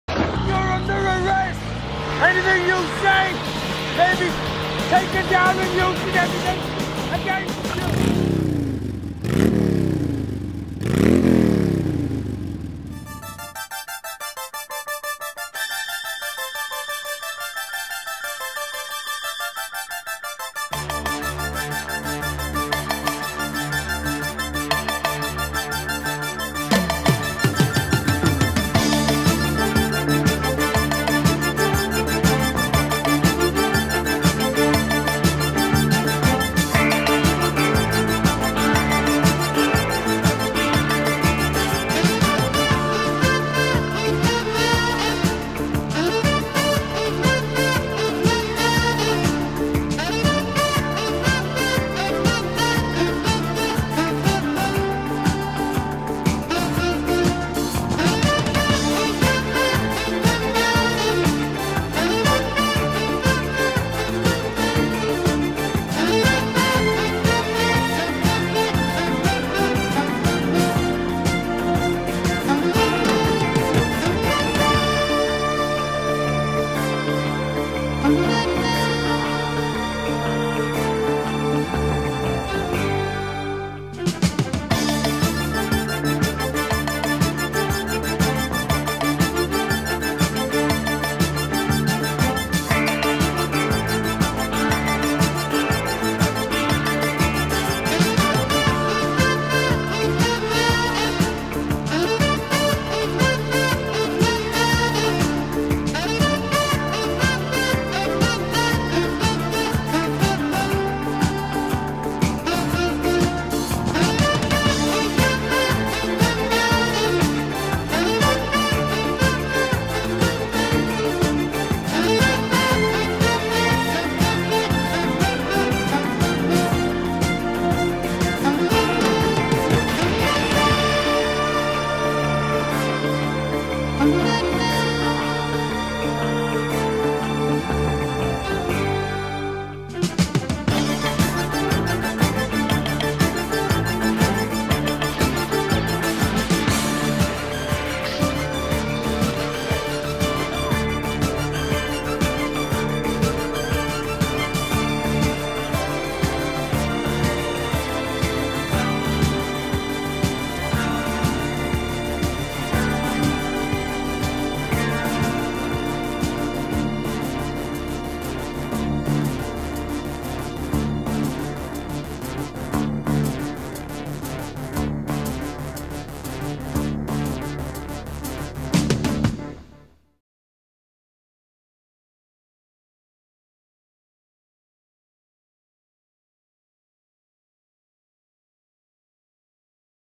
ROY ALON STUNT REEL
Roy Alon was one of the great stunt performers - here is a collection of some of my favourite moments of his, worked into a reel.